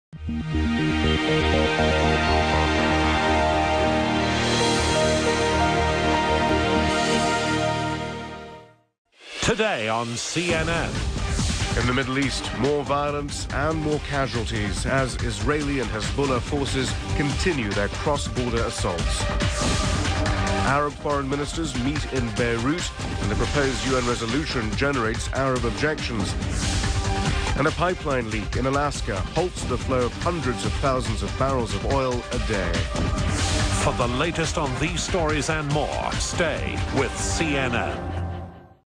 CNN International Lineups (2006)
A short "Up Next" bumper, followed by a lineup of today's coverage. From CNN International.